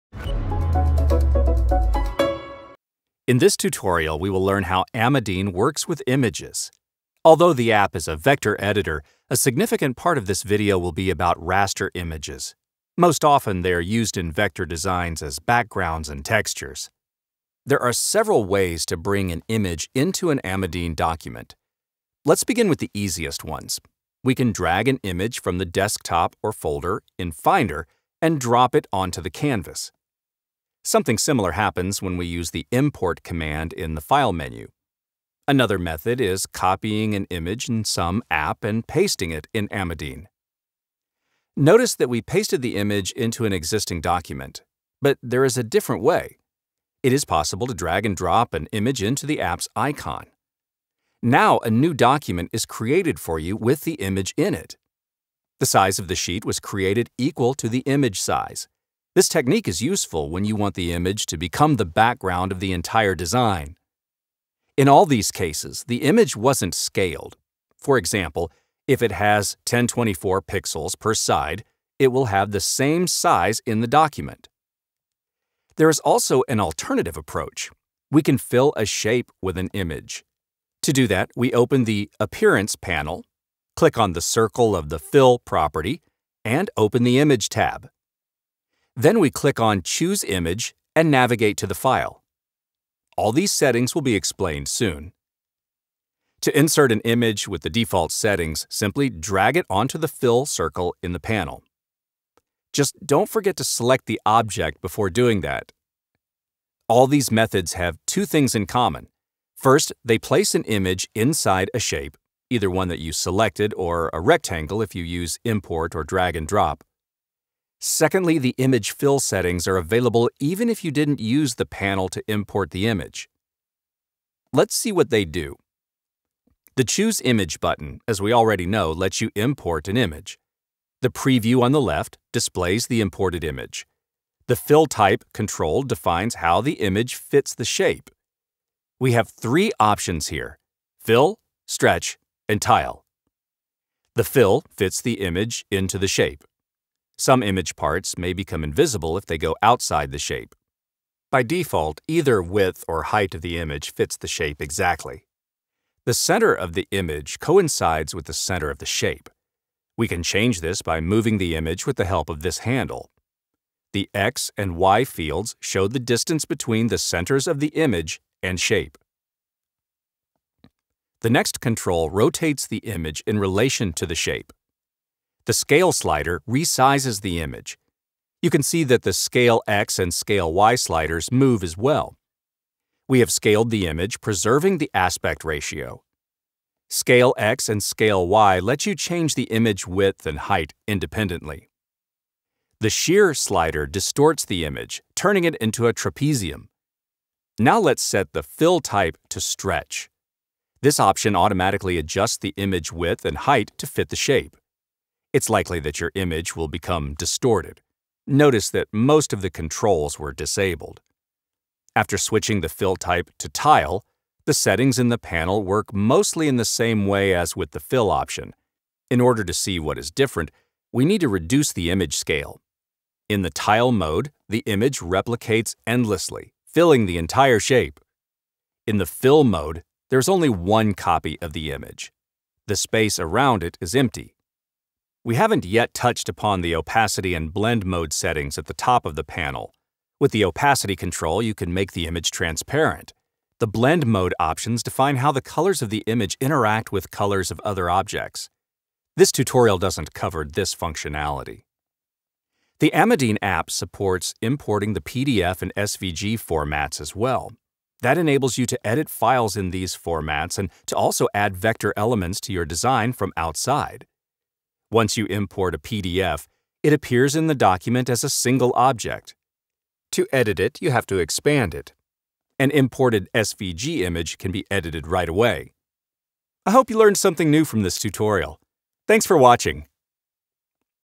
Working With Images—Part 1—Amadine for Mac Tutorials
This tutorial will shed the light upon how Amadine for Mac works with images.

To learn more on the topic, there’s an article dedicated exactly to filling a shape with an image or photo:

Timecodes:
0:00 Intro
0:17 Several ways to bring an image into the Amadine app.
1:15 Filling a shape with an image.
2:04 The image Fill settings.
2:14 The Fill type control and its setting options. Fill, Stretch, Tile.
3:55 The Opacity and Blend mode settings.
4:12 SVG and PDF import.